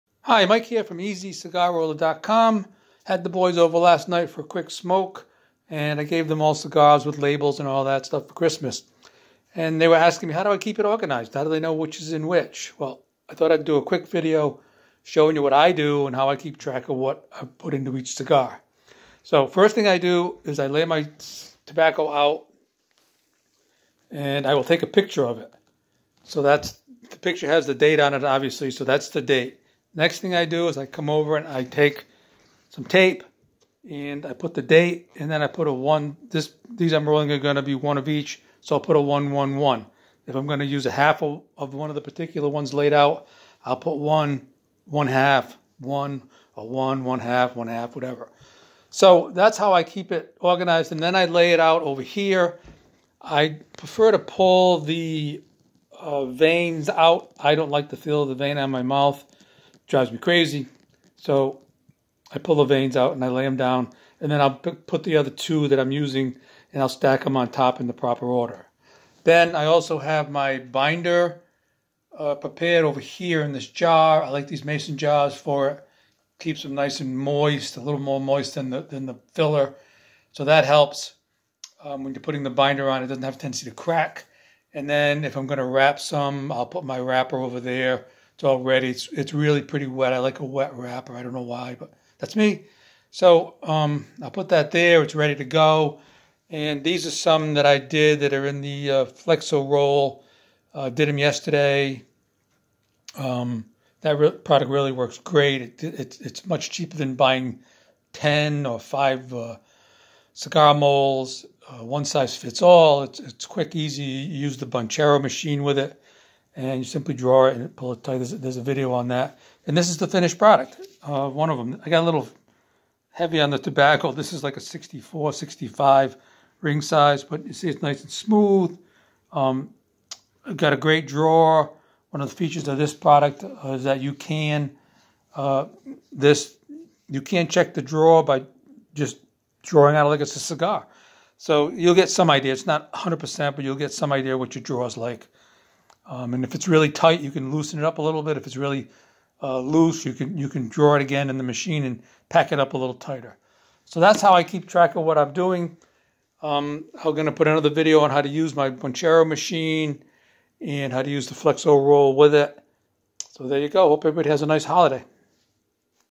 PREPPING FOR CIGAR ROLLING AT HOME
A quick way to keep track of your cigar blends when making cigars at home.